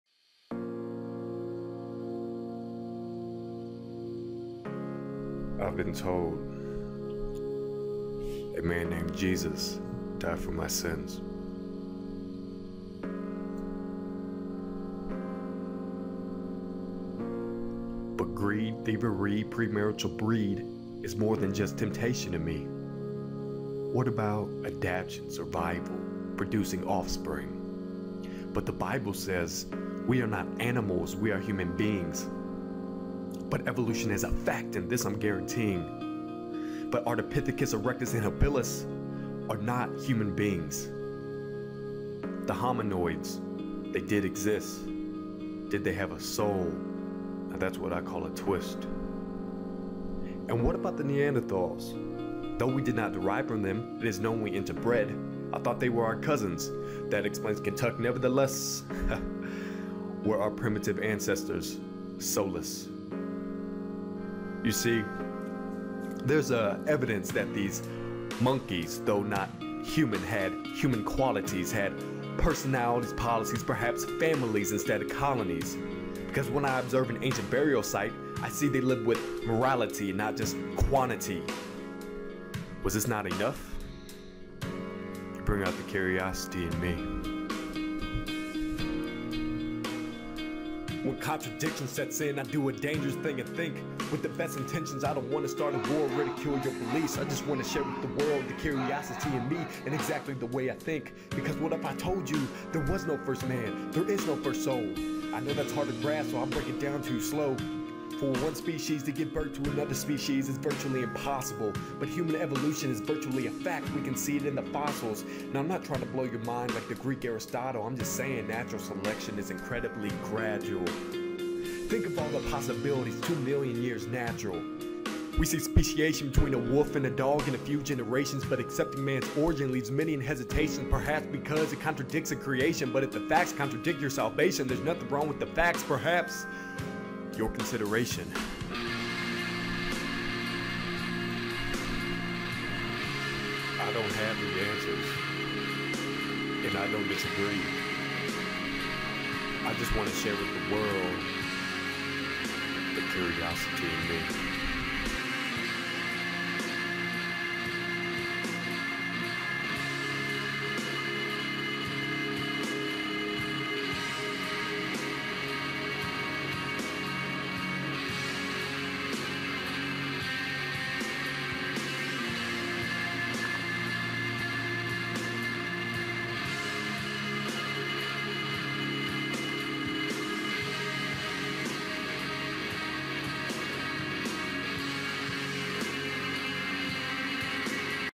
The Curiosity In Me
This poem is an extended hand of my passion for science and the natural processes of the universe. Science is both whole and real and we must cherish the known and admire the unknown. Science is real no matter who believes in it.

Script:

You Bring Out the Curiosity in Me  Dylan Gloy

I've been a man named told Jesus died for my sins,

but greed, thievery, premarital breed, is more than just temptation to me.

What about adaption, survival, producing offspring?

But the Bible says, "We are not animals, we are human beings."

But evolution is a fact; 'N this I'm guaranteeing.

But ardipithecus, erectus, and habillis… are not human beings..

The hominids, they did exist.

Did they have a soul? Now, that's what I call a twist.

And what about the Neanderthals?

Though we did not derive from them, it is known we interbred.

I thought they were our cousins? This explains Kentuck.. Nevertheless ha

Were our primitive ancestors soulless?

You see, there's "ugh" evidence that these "monkeys"

Though not human, had human qualities.

Had personalities, policies, and perhaps families instead of colonies.

Because when I observe an ancient burial site, I see they lived with morality and not just

quantity….. was this not enough?

You bring out the curiosity in me.

When contradiction sets in, I do a dangerous thing and think,

with the best intentions. I don't want to start a war or ridicule your beliefs.

I just want to share with the world the curiosity in me, and exactly the way I think.

Because what if I told you…

There was no first man, there is no first soul?..

I know this is hard to grasp but I'll break it down to you slow.

For one species to give birth to another species is virtually impossible.

But human evolution is virtually a fact; we can see it in the fossils.

Now, I'm not trying to blow your mind like the Greek Aristotle.

I'm just saying, natural selection is incredibly gradual.

Think of all the possibilities 2 million years natural.

We see speciation between a wolf and a dog in a few generations.

But accepting man's origin leaves many in hesitation.

Perhaps it's because it contradicts a creation.

But if the facts contradict your salvation, there's nothing wrong with the facts, perhaps..

your consideration.

I don’t have the answers, I don’t disagree.

I just want to share with the world.. the curiosity in me.